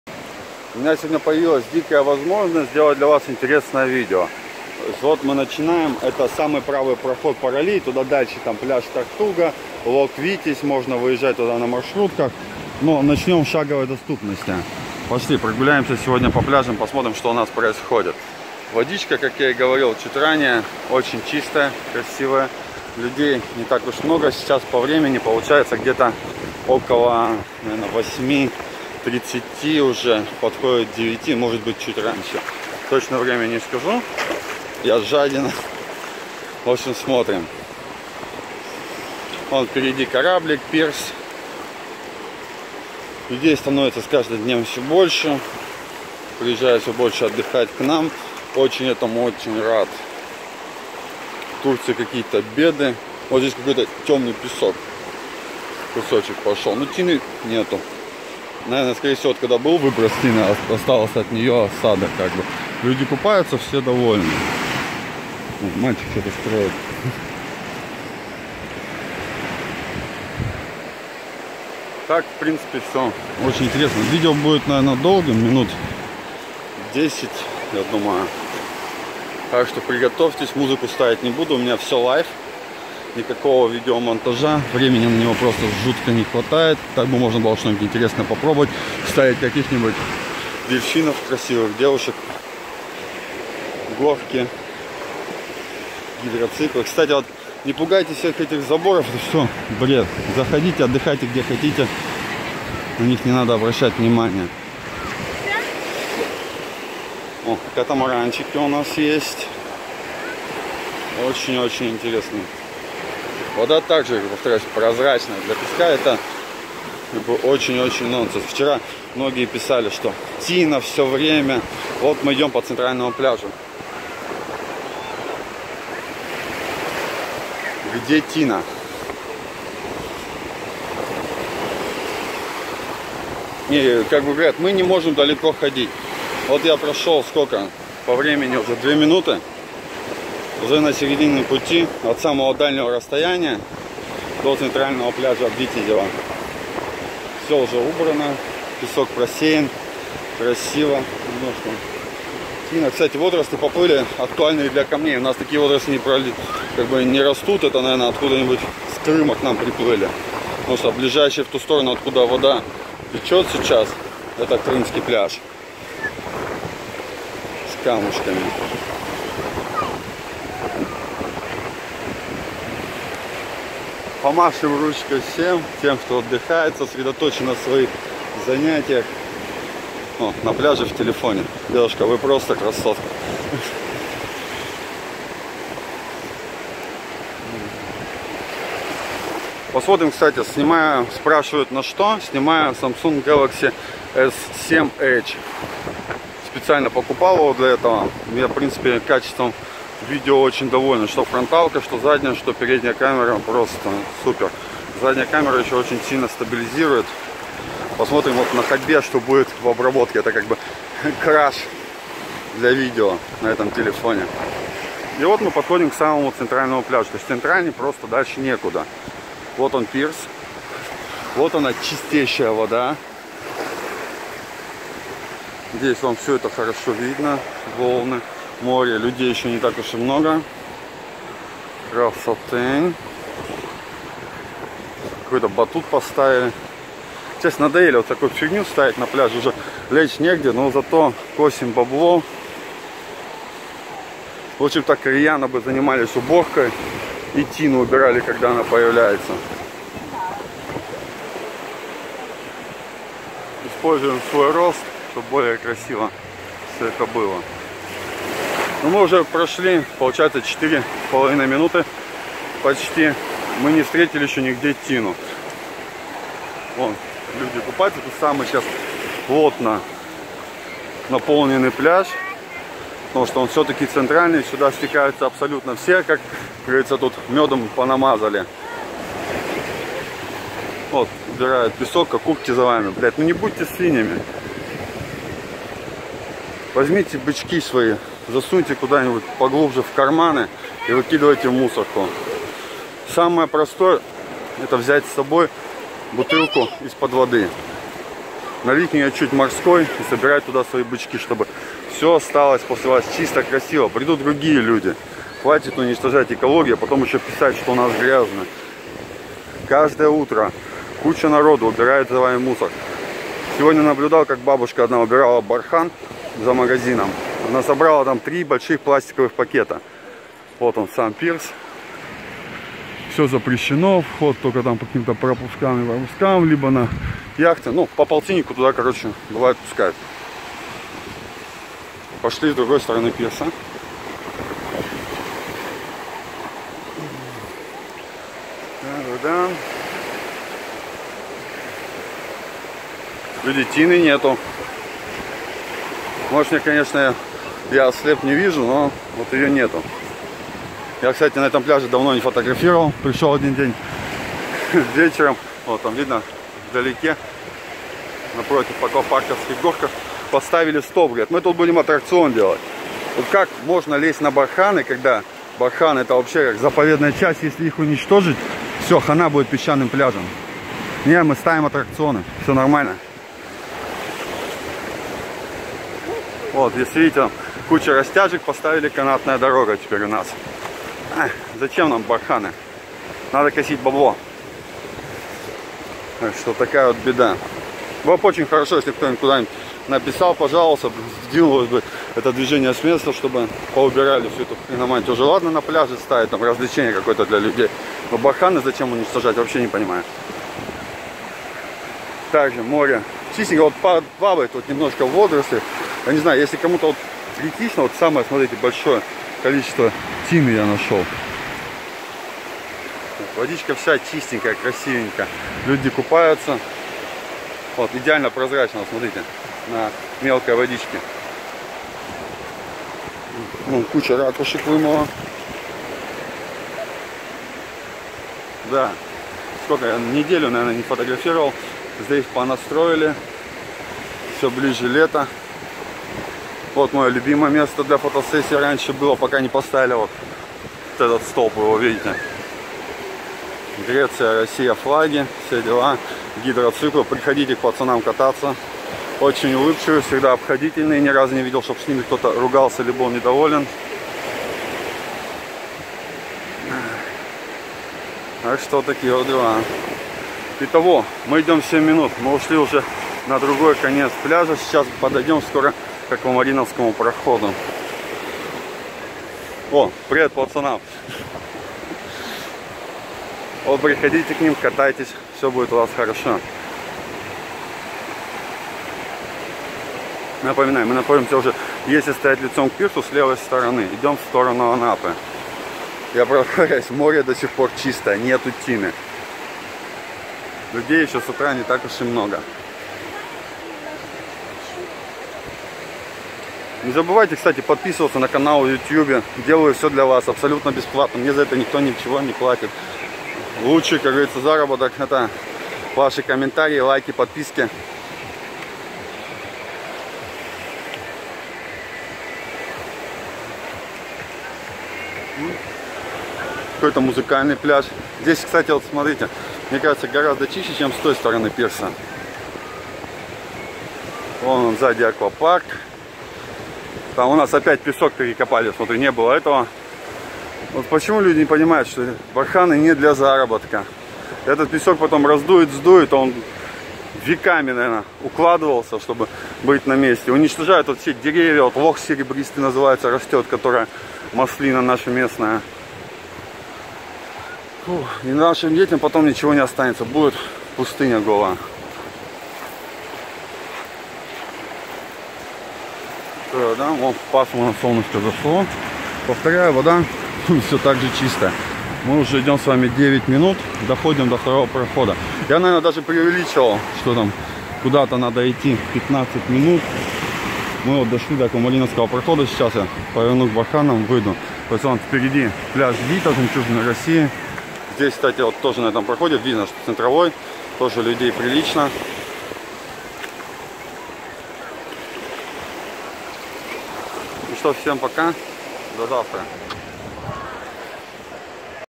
Прогулка по пляжу Витязево (Анапа) в поисках тины на Черном Море 22.07.2016
Начал свою прогулку от левого прохода с улицы мира и до аквамариновского прохода, в конце июля людей было еще не очень много, как и тины в  черном море.

Прошу вас добавляйтесь в друзья буду отвечать на все ваши воросы.